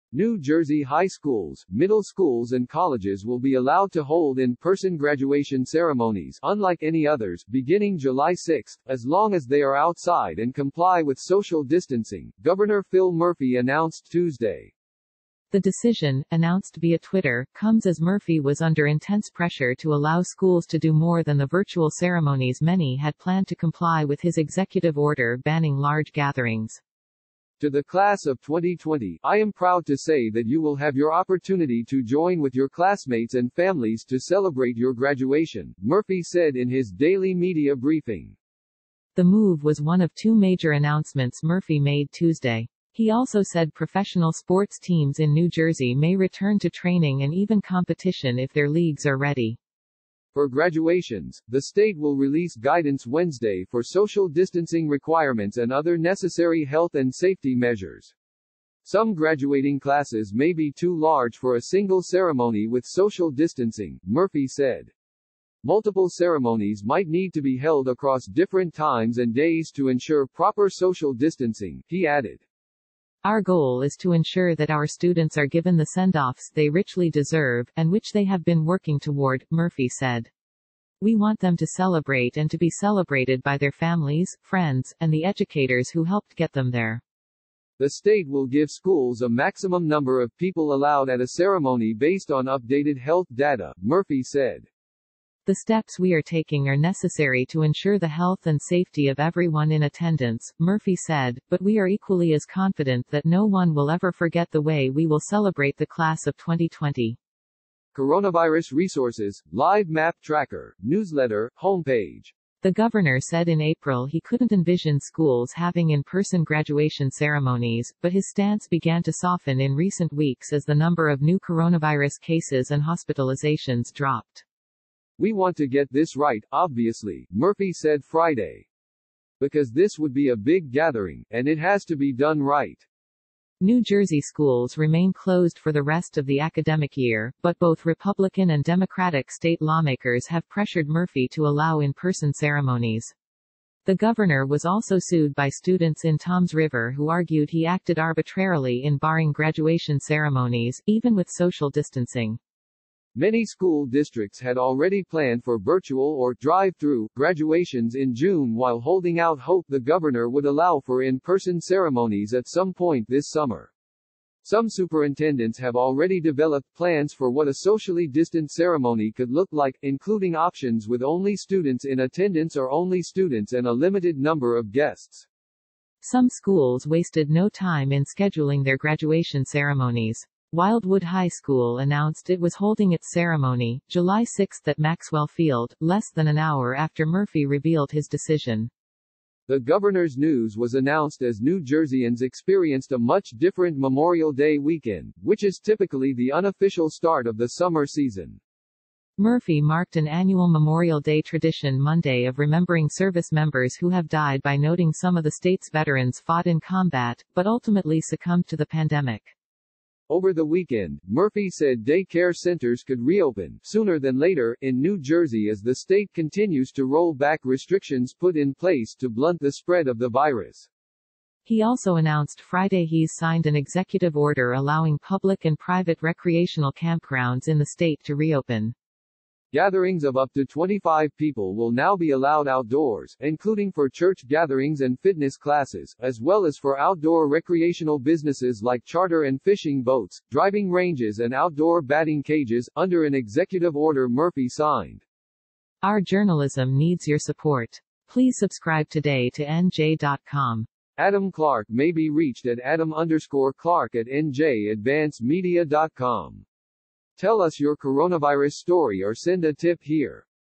N.J. schools can have in-person graduations in July in major change to coronavirus rules, Murphy s
New Jersey high schools, middle schools and colleges will be allowed to hold in-person graduation ceremonies “unlike any others” beginning July 6, as long as they are outside and comply with social distancing, Gov. Phil Murphy announced Tuesday.